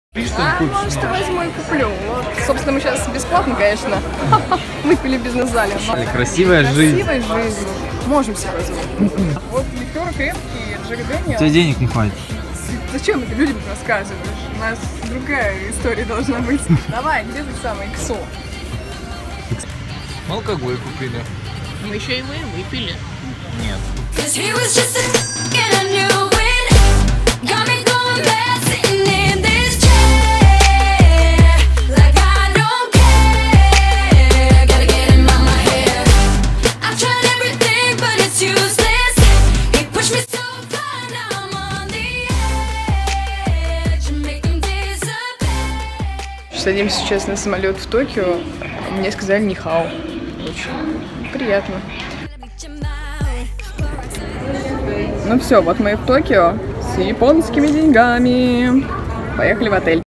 Нихао! Летим в Токио бюджетно!
DutyFree. Рассвет над Китаем. Пересадка в Пекине. Первые секунды в Токио!